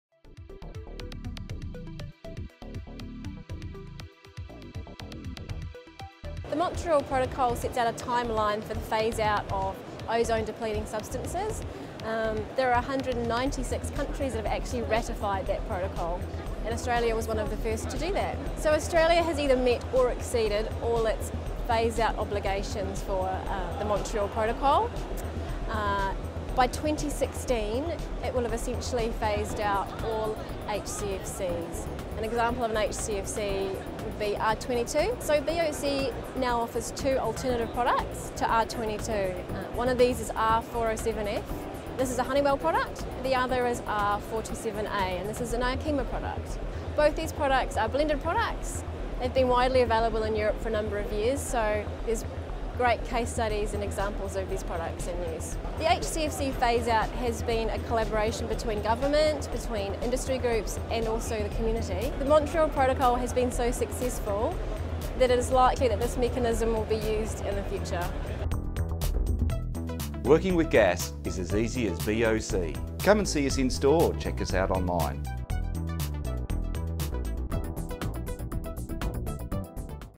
BOC - The Montreal Protocol
BOC and the Montreal Protocol

The Montreal Protocol outlines a timeline for the phase out of ozone depleting substances. Australia is one of the 196 countries which have ratified the protocol.

By 2016 Australia will have essentially phased out all HCFCs. R22 is an example of a HCFC which will be phased out.

In this video, BOC’s Refrigerant Market Manger speaks about the Montreal Protocol and the impact on R22.

BOC now offers 2 alternatives to R22: PERFORMAXTM R407F and FORANE® R427A.

Both these products are blended products and have been widely available in Europe for several years.